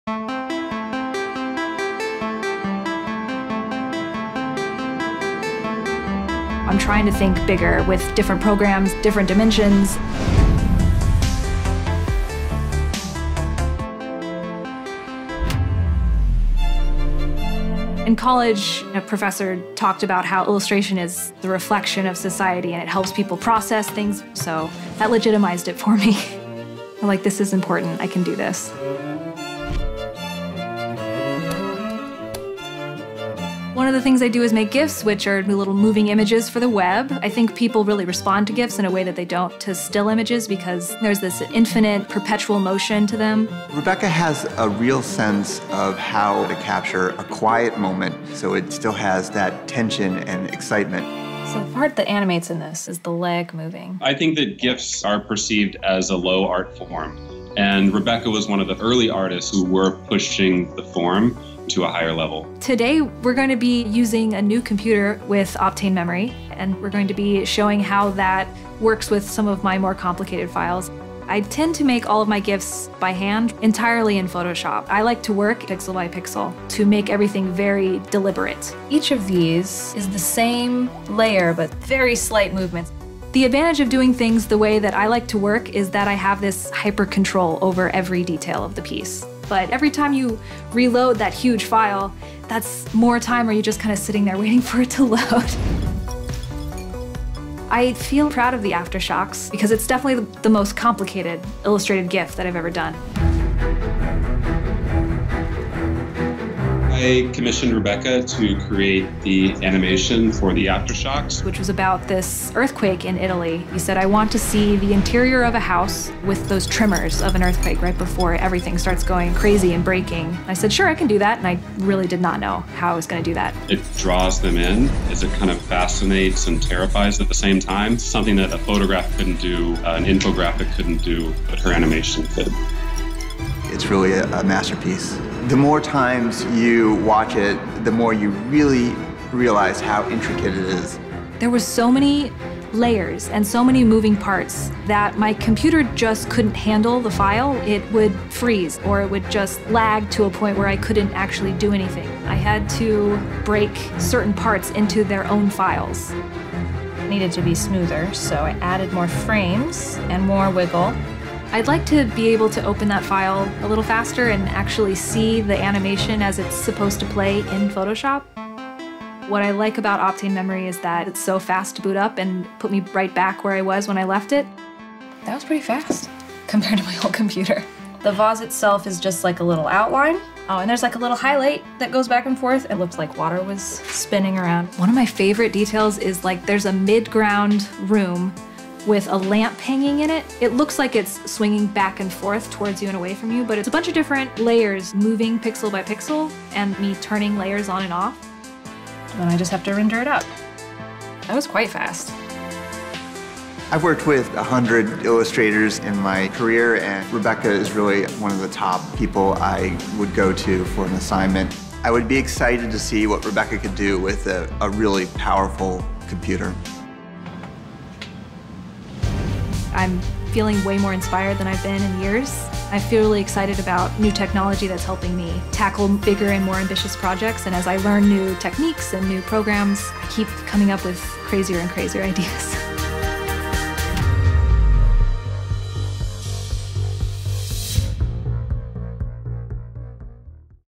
You Need To See This Hand-Drawn Animated GIF
Thanks to Intel Optane memory, Rebecca Mock is now able to animate her hand-drawn gifs faster than ever. Watch how this technology saves her time and stress as she creates her famous 12 frame GIF to commemorate an earthquake.